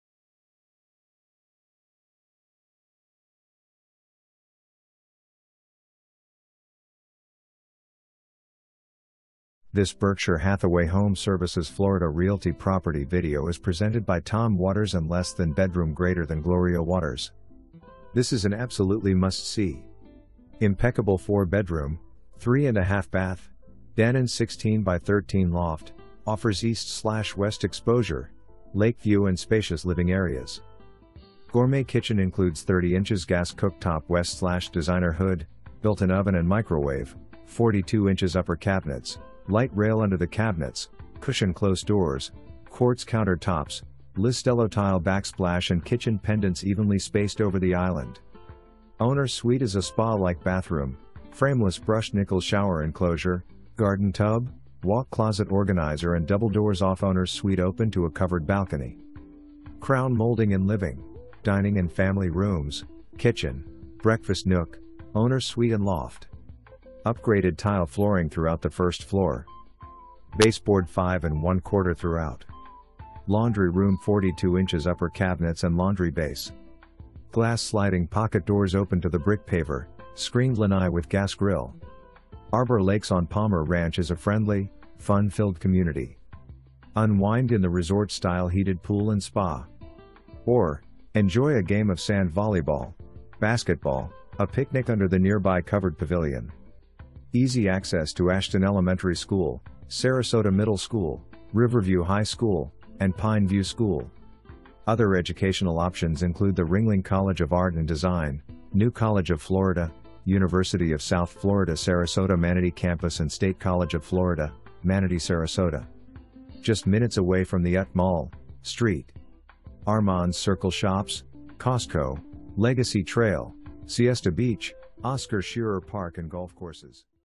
Berkshire Hathaway HomeServices Florida Realty - 5954 ANISE DRIVE
This is an absolutely must see. Impeccable 4 bedroom, 3 1/2 bath, den and 16 x 13 loft, offers east/west exposure, lake-view and spacious living areas. Gourmet kitchen includes 30" gas cooktop w/designer hood, built in oven and microwave, 42" upper cabinets, light rail under the cabinets, cushion close doors, Quartz counter tops, Listello tile backsplash and kitchen pendants evenly spaced over the island. Owner’s suite is a spa-like bathroom, frameless brushed nickel shower enclosure, garden tub, walk-closet organizer and double doors off owner’s suite open to a covered balcony. Crown molding in living, dining and family rooms, kitchen, breakfast nook, owner’s suite and loft. Upgraded tile flooring throughout the first floor. Baseboard 5 ¼” throughout. Laundry room 42" upper cabinets and laundry base. Glass sliding pocket doors open to the brick paver, screened lanai with gas grille. Arbor Lakes on Palmer Ranch is a friendly, fun-filled community. Unwind in the resort-style heated pool and spa. Or, enjoy a game of sand volleyball, basketball, a picnic under the nearby covered pavilion. Easy access to Ashton Elementary School, Sarasota Middle School, Riverview High School and Pine View School. Other educational options include the Ringling College of Art and Design, New College of Florida, University of South Florida Sarasota-Manatee Campus and State College of Florida, Manatee-Sarasota. Just minutes away from The UTC Mall, St. Armand’s Circle Shops, Costco, Legacy Trail, Siesta Beach, Oscar Scherer Park and golf courses.

Tom Waters and Gloria Waters
Berkshire Hathaway HomeServices Florida Realty